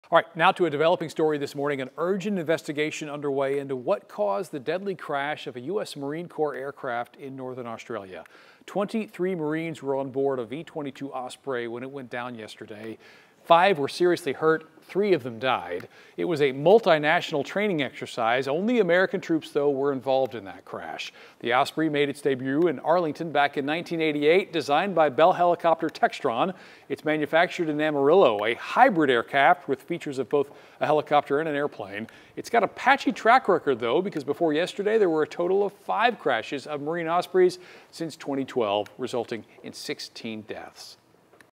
Australia plane crash: 3 US Marines killed, more injured during training drill
The Osprey, a hybrid aircraft that takes off and lands like a helicopter, crashed into a tropical forest and burst info flame. It's unclear what caused the crash.